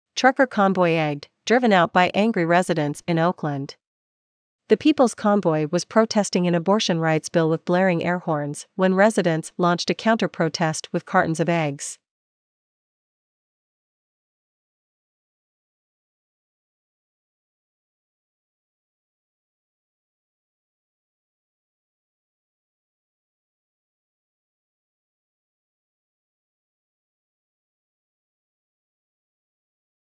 Trucker convoy egged, driven out by angry residents in Oakland
The People's Convoy was protesting an abortion rights bill with blaring airhorns when residents launched a counter-protest with cartons of eggs.